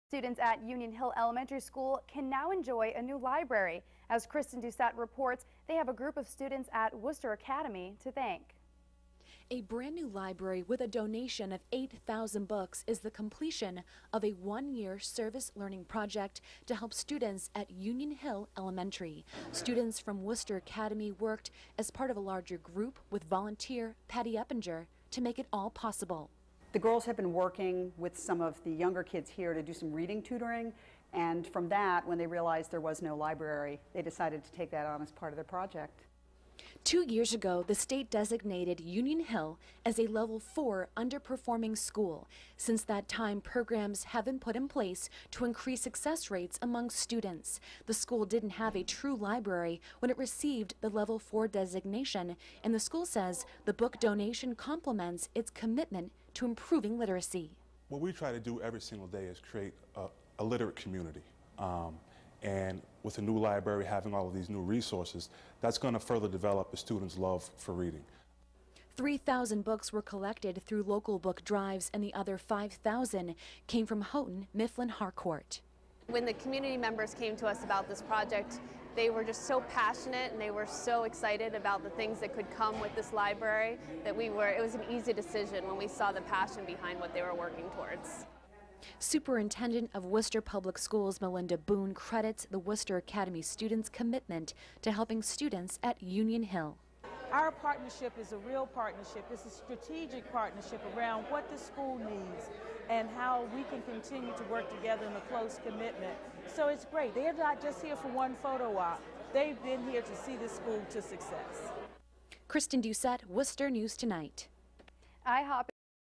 NECN Profile Leadership of Worcester Academy's Grade 8 Girls
This Worcester Channel 3 news story profiles the contributions of five Worcester Academy girls, all members of Worcester Academy Middle School's global award-winning Destination Imagination team, whose vision led to the creation of a new library at nearby Union Hill School. Achieving the Honorable.